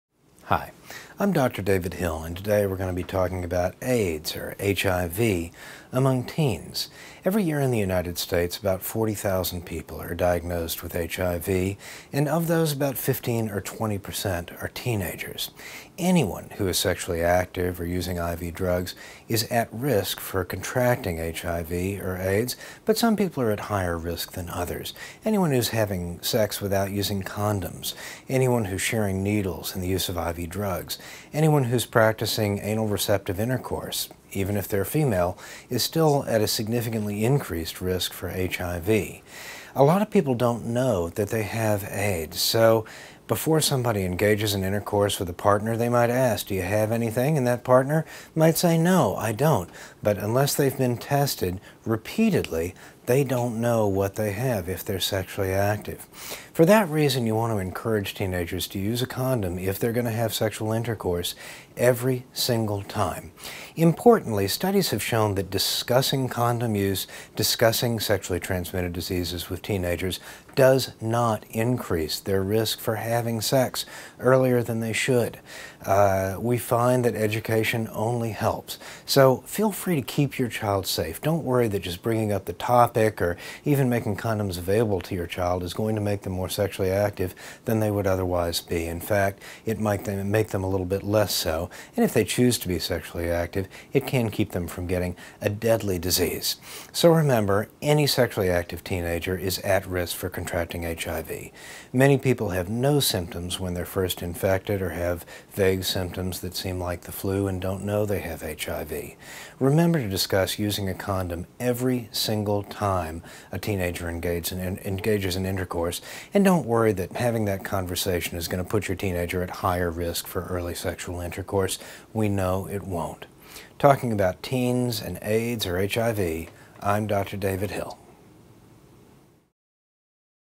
Autism & Pediatric Diseases : About AIDS Among Teens
About 15 percent to 20 percent of people who are diagnosed with HIV every year are teenagers, and anyone who is sexually active or using IV drugs is at risk for contracting HIV or AIDS. Find out why education of teenagers is so important in the prevention of AIDS with help from a practicing pediatrician in this free video on pediatrics and AIDS among teenagers.